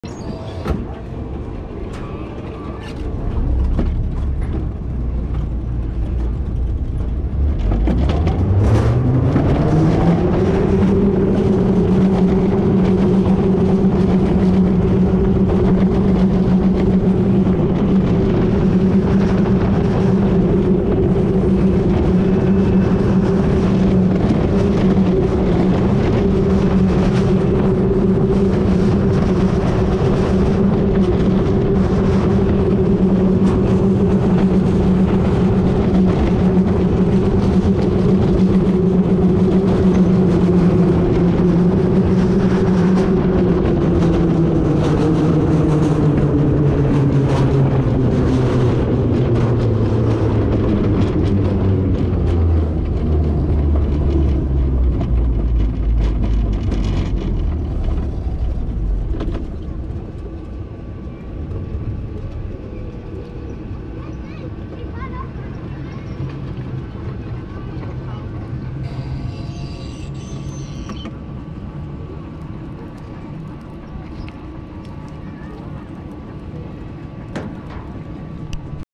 Bob Kart POV, Schwaben Park Wiegand Powered Attraction | Non-Copyright
Bob Kart is a Wiegand creation at Schwaben Park.  This is the powered version of a mountain coaster.  This POV shows what happens when you go full speed through the entire layout.